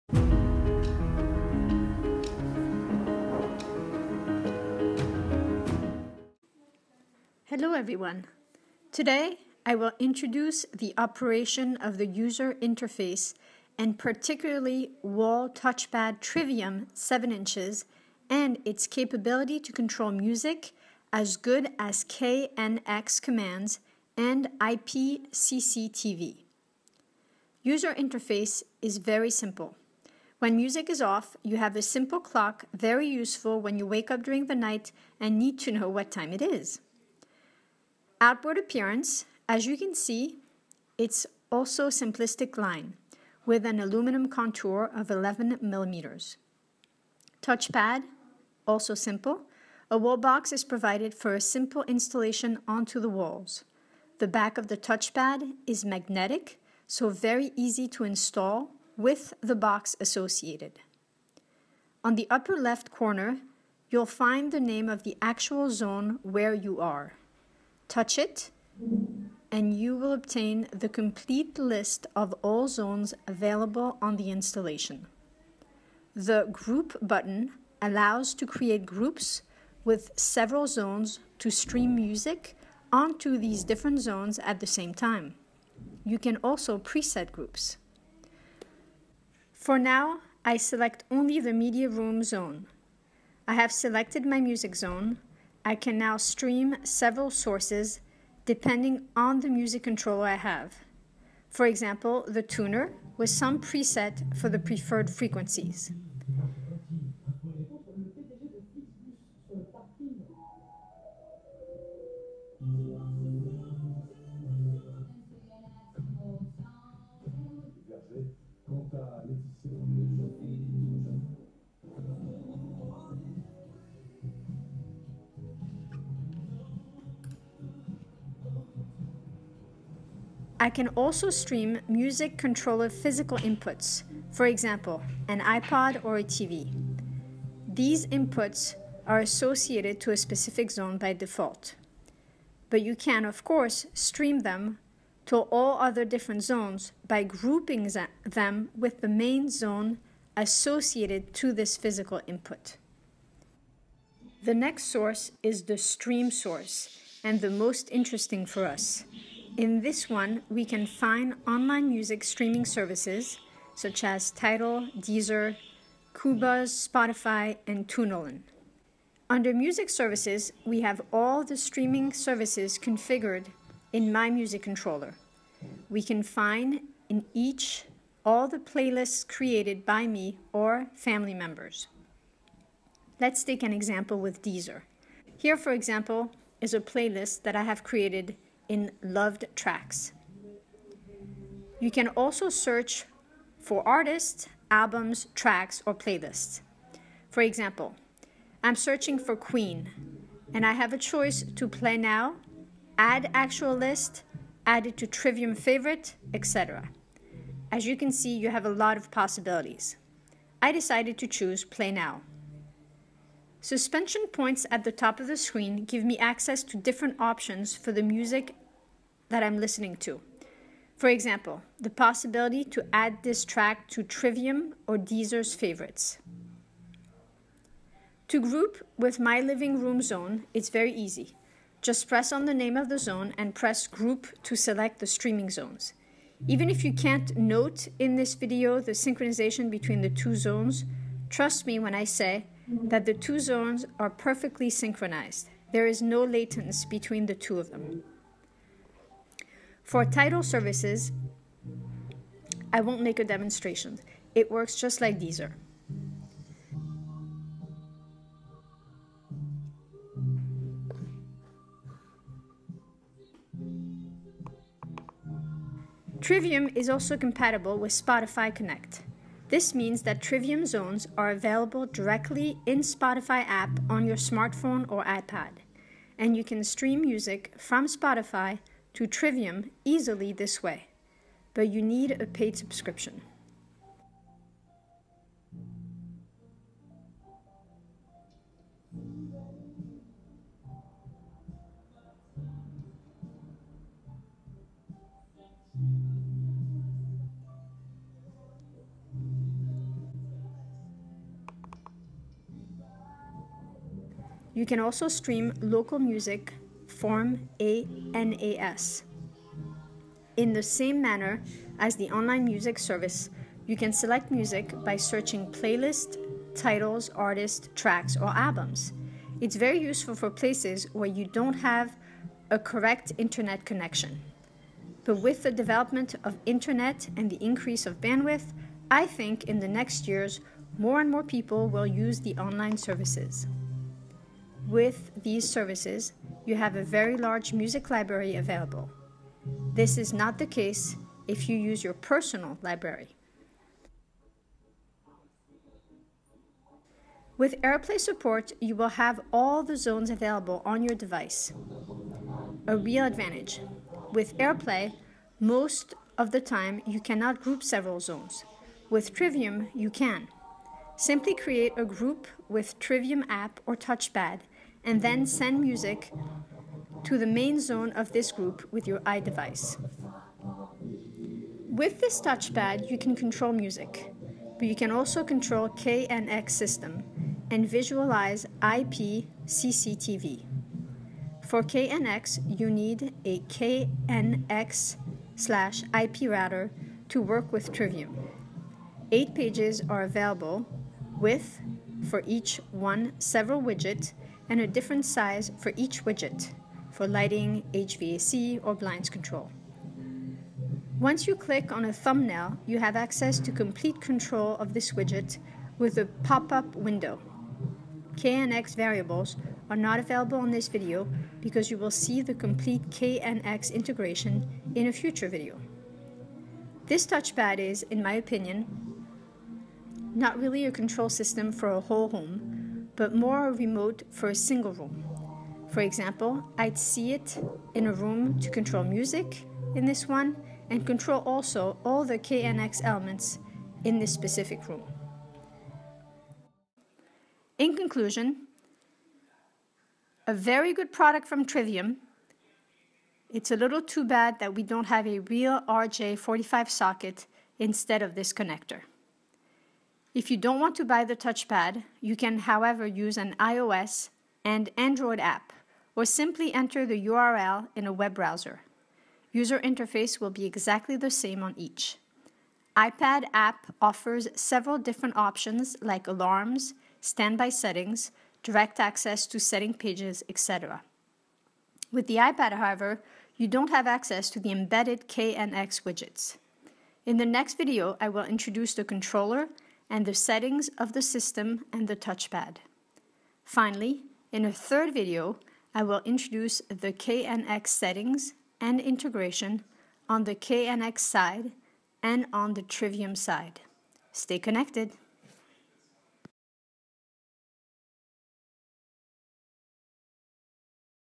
Trivum Touchpad Introduction English
Introduction to Trivum Music Center v9, on touchpad and iOS app, and with the dedicated 7" Trivum touchpad. Complete article is available: